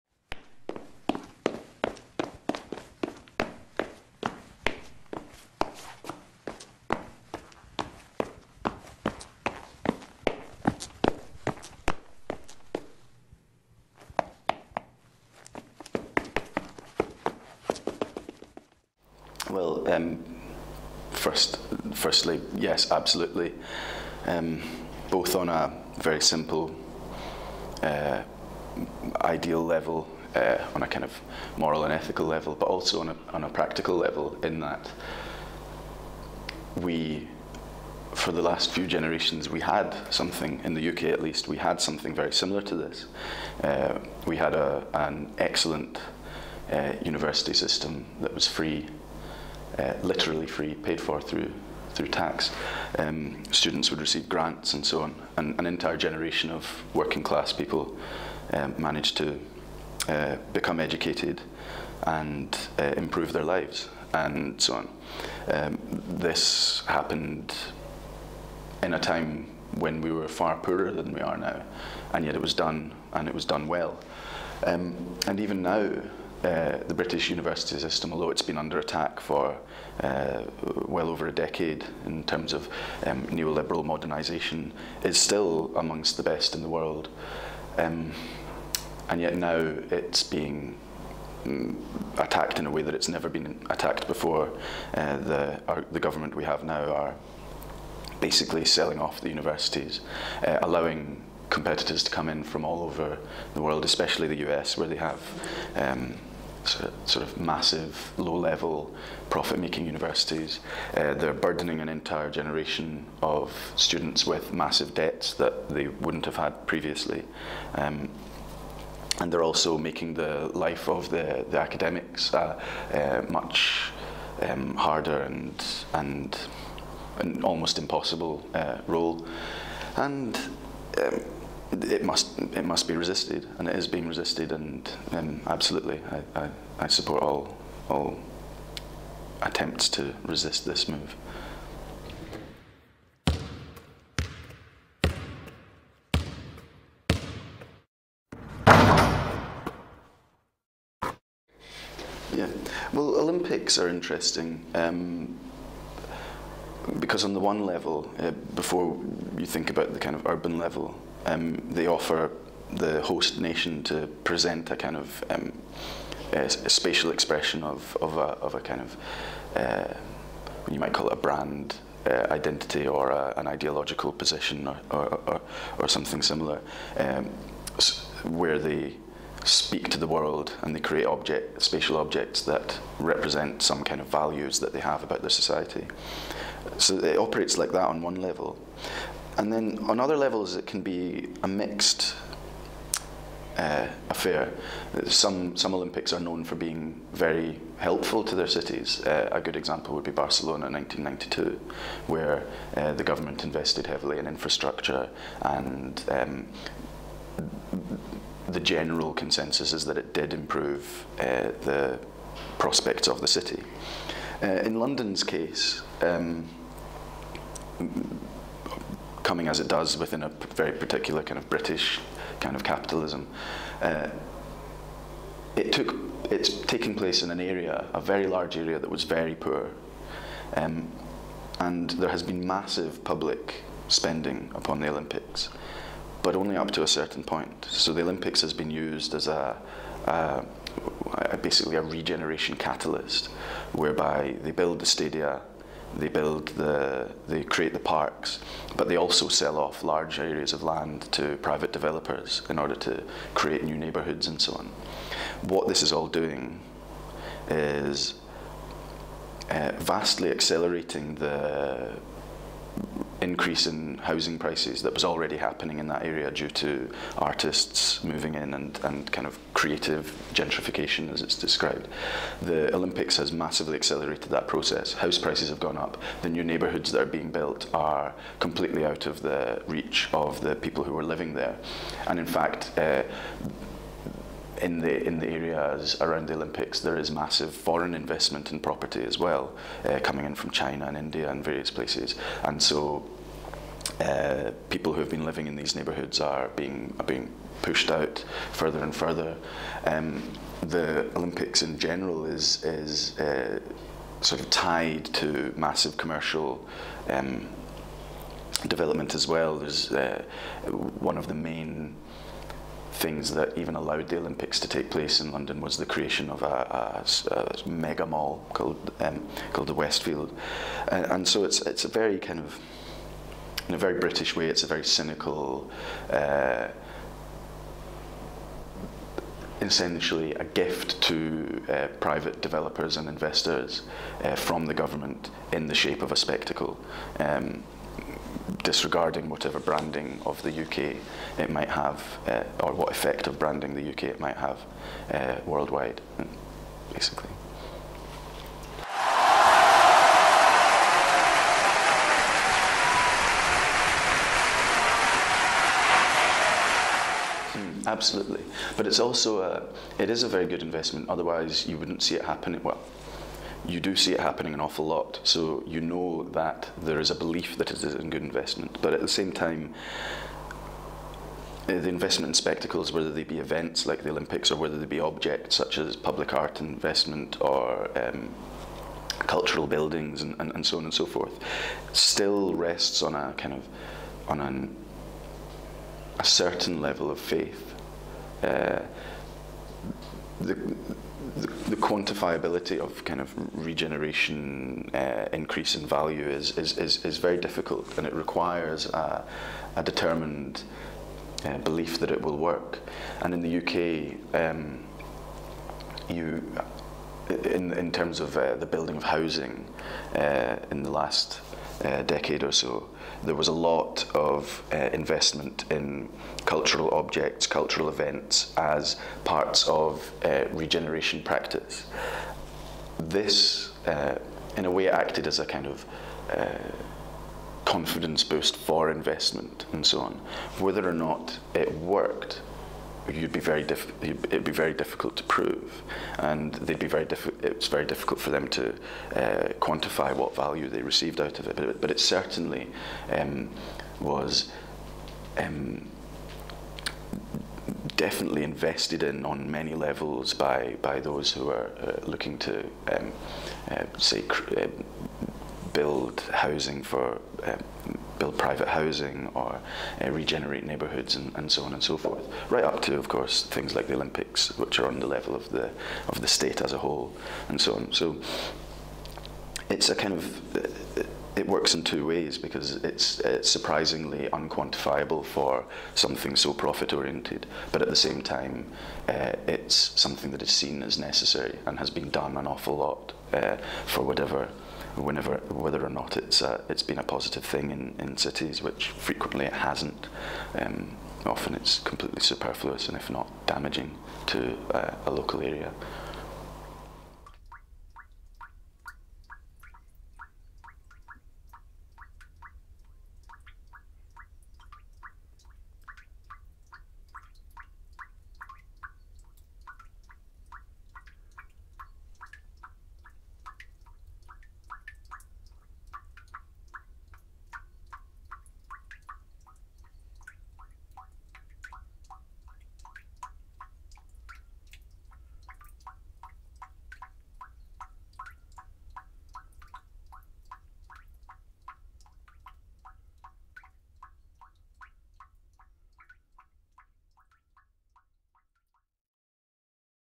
Douglas Murphy: Free education, London Olympics and investment in spectacles /// Zagreb 2011
Douglas Murphy was interviewed in November 2011, during his stay in Zagreb where he held the lecture "Monument to What?" ) in net.culture club mama (mi2.hr), as a part of cdu.hr 2011 "Methodologies of Valorization" program.